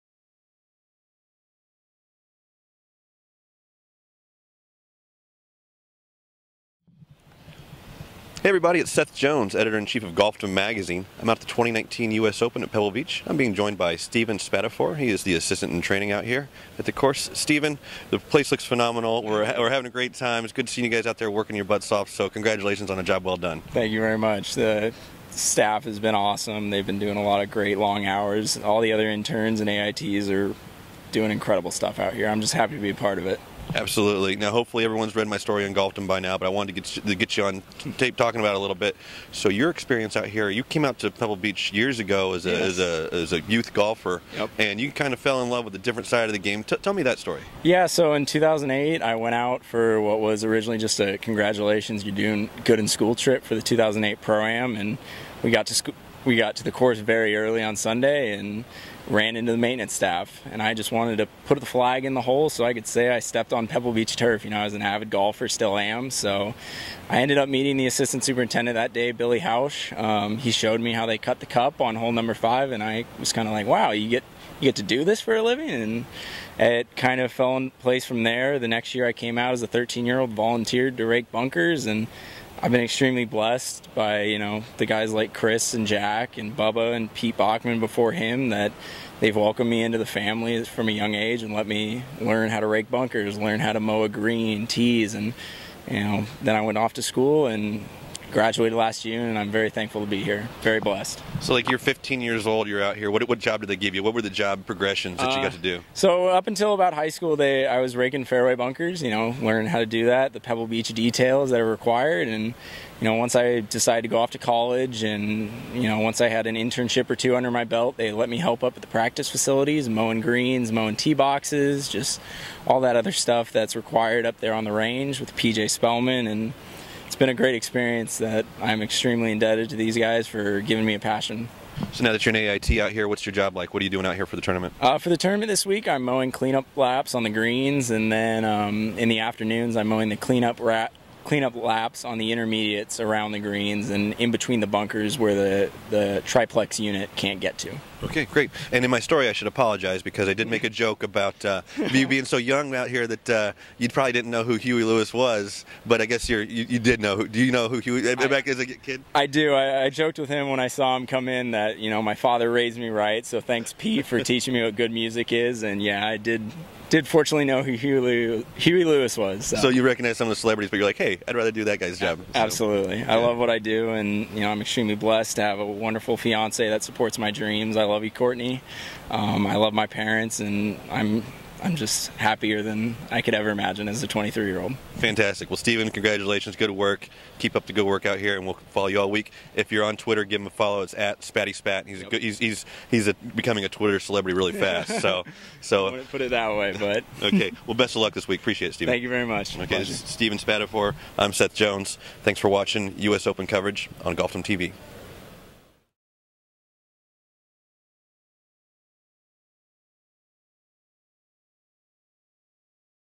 AIT Steven Spatafore’s journey to Pebble Beach
Steven Spatafore, Pebble Beach Assistant-in-Training, shares the story of how a trip to Pebble Beach as a kid set him on the path to where he is today.